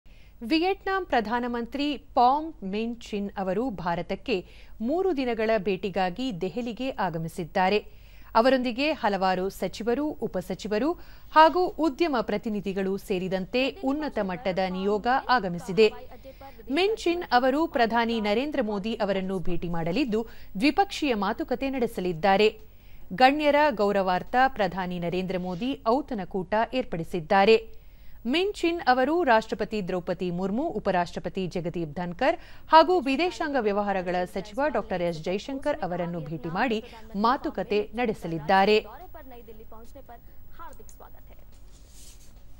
3 ದಿನಗಳ ಭಾರತ ಪ್ರವಾಸದಲ್ಲಿ ವಿಯೆಟ್ನಾಂ ಪ್ರಧಾನಿ ; ಪ್ರಧಾನಿ ನರೇಂದ್ರ ಮೋದಿ ಅವರೊಂದಿಗೆ ಸಮಾಲೋಚನೆ
ವಿಯೆಟ್ನಾಂ ಪ್ರಧಾನ ಮಂತ್ರಿ  ಪಾಮ್ ಮಿನ್ ಚಿನ್ (Pham Minh Chinh) ಅವರು ಭಾರತಕ್ಕೆ  ಮೂರು ದಿನಗಳ ಭೇಟಿಗಾಗಿ  ದೆಹಲಿಗೆ ಆಗಮಿಸಿದ್ದಾರೆ. ಅವರೊಂದಿಗೆ   ಹಲವಾರು ಸಚಿವರು, ಉಪಸಚಿವರು ಹಾಗೂ  ಉದ್ಯಮ ಪ್ರತಿನಿಧಿಗಳು ಸೇರಿದಂತೆ  ಉನ್ನತ ಮಟ್ಟದ ನಿಯೋಗ ಆಗಮಿಸಿದೆ.
ಮಿನ್ ಚಿನ್ ಅವರು ಪ್ರಧಾನಿ ನರೇಂದ್ರ ಮೋದಿ ಅವರನ್ನು ಭೇಟಿ ಮಾಡಲಿದ್ದು, ದ್ವಿಪಕ್ಷೀಯ  ಮಾತುಕತೆ ನಡೆಸಲಿದ್ದಾರೆ.
ಗಣ್ಯರ ಗೌರವಾರ್ಥ ಪ್ರಧಾನಿ ನರೇಂದ್ರ ಮೋದಿ ಔತಣಕೂಟ  ಏರ್ಪಡಿಸಿದ್ದಾರೆ.
ಮಿನ್ ಚಿನ್ ಅವರು ರಾಷ್ಟ್ರಪತಿ ದ್ರೌಪದಿ ಮುರ್ಮು, ಉಪರಾಷ್ಟ್ರಪತಿ ಜಗದೀಪ್  ಧನಕರ್  ಹಾಗೂ ವಿದೇಶಾಂಗ ವ್ಯವಹಾರ ಸಚಿವ ಡಾ. ಎಸ್. ಜೈಶಂಕರ್ ಅವರನ್ನು ಭೇಟಿ ಮಾಡಿ ಮಾತುಕತೆ ನಡೆಸಲಿದ್ದಾರೆ.